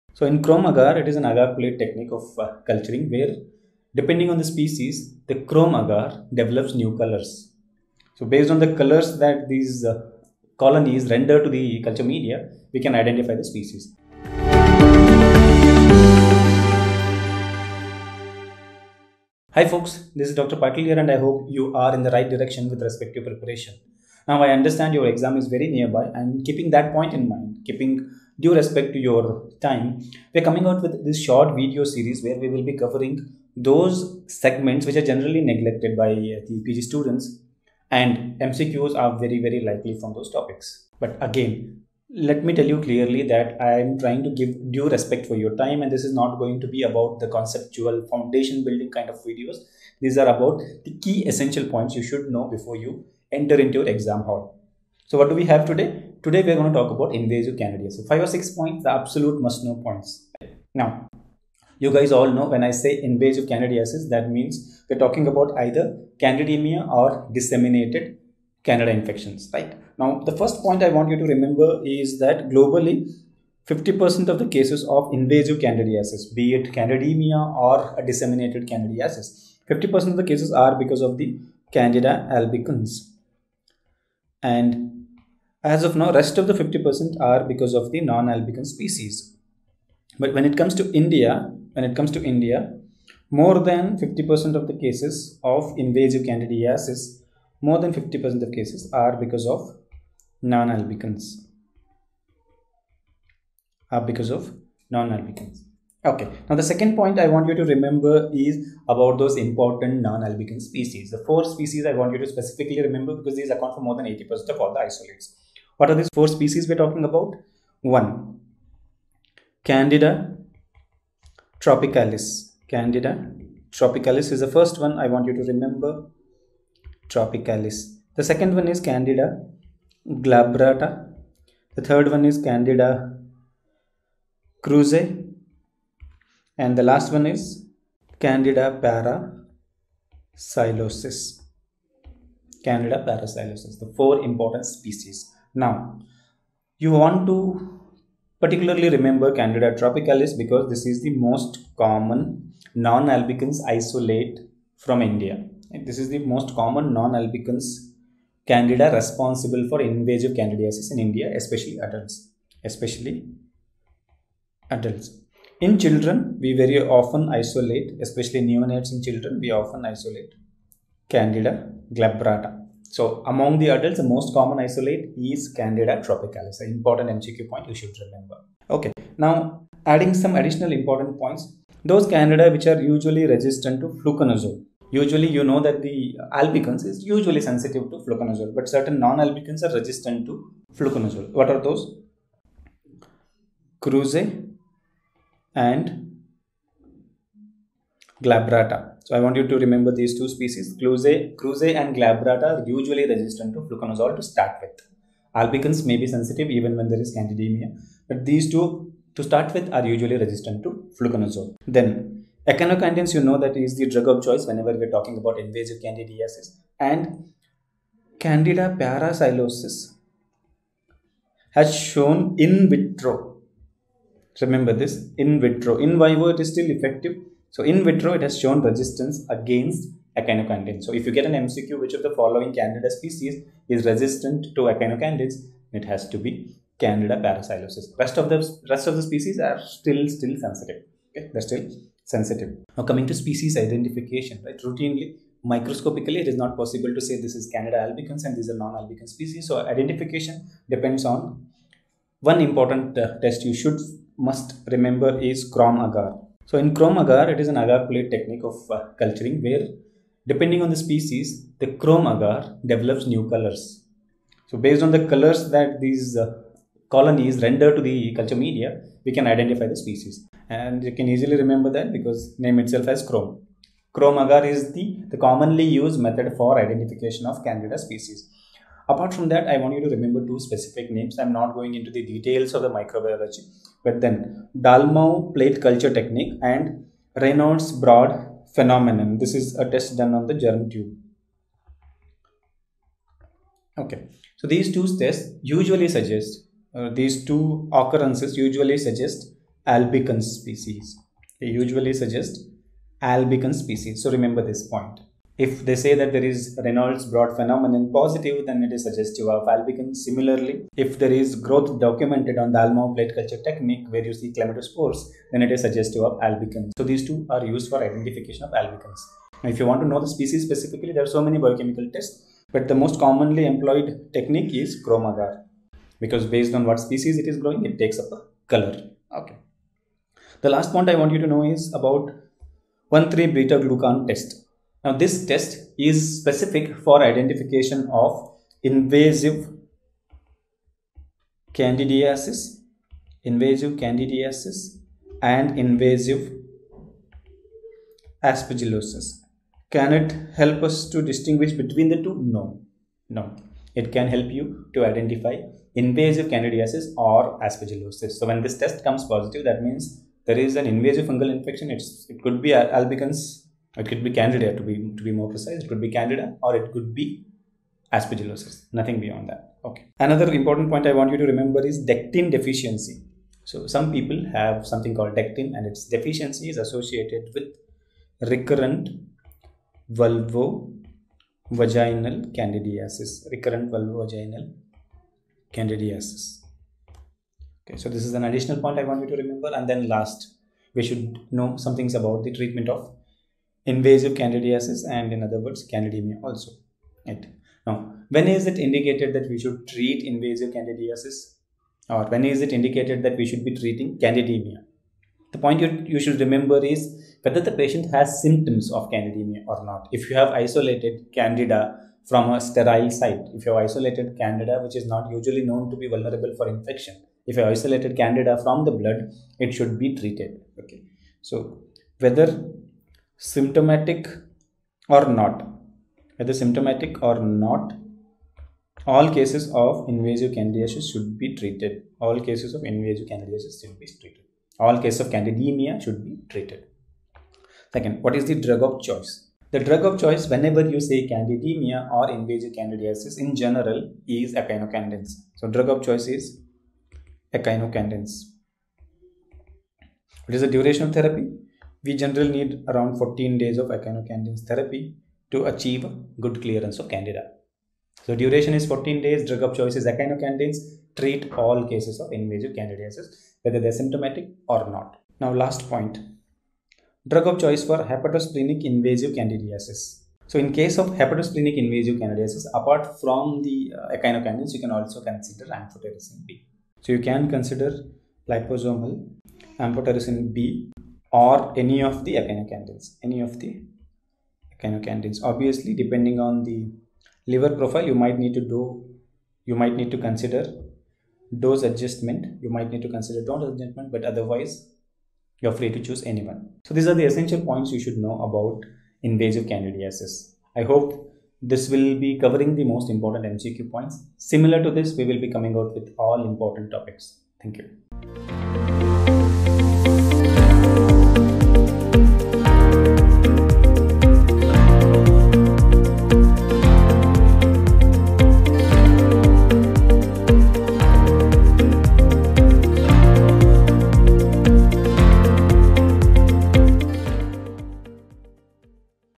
Invasive Candidiasis | Dr. Santosh M Patil | General Medicine | NEET SS
Watch this insightful session by Dr. Santosh M Patil, where he discusses everything you need to know about Invasive Candidiasis. If you have any questions, ask us in the comment section below.